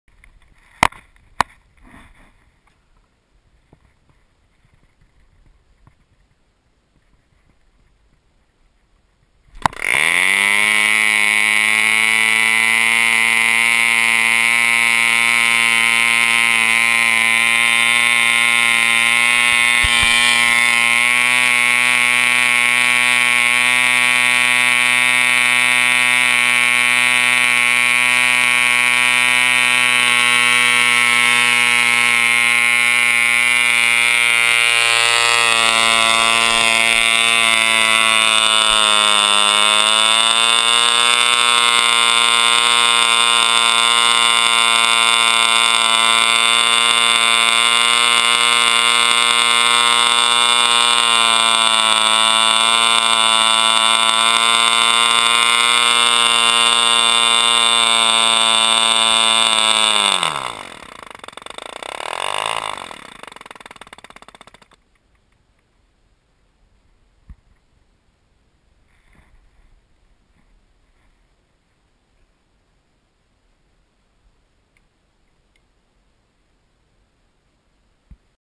My Old Super 60
Running on Nitro fuel and glow ignition. This vid is an experiment to test the difference between standard picture resolution and high definition. Now I see that there is no comparison but uploading HD is a pain.